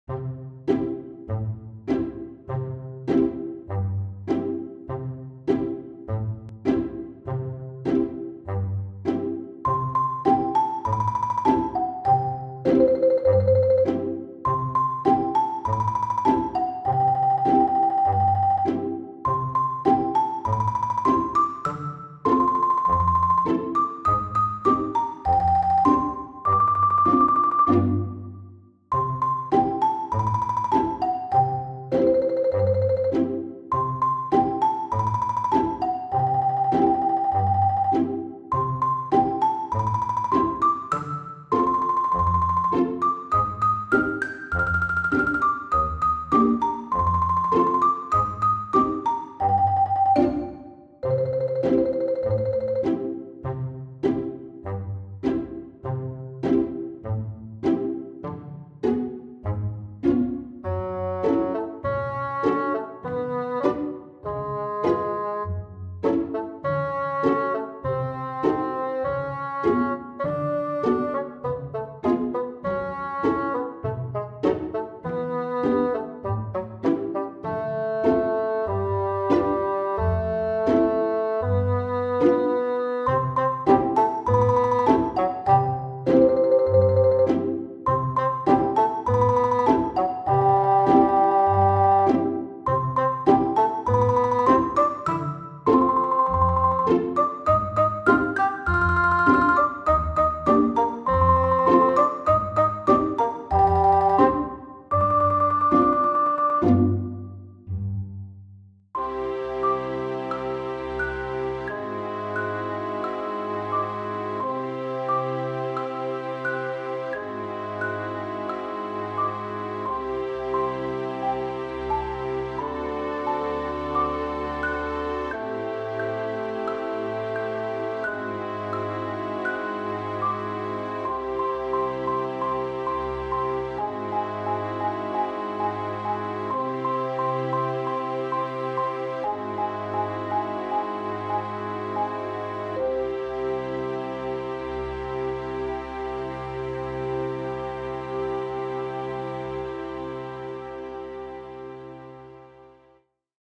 Music Theory and Composition 3: Final Project
This piece was written as a final project and was required to contain the following: an AABA overall form, a modulation to a distant key, at least one substitute dominant chord, at least one modal interchange chord, and at least one secondary dominant diminished 7th chord.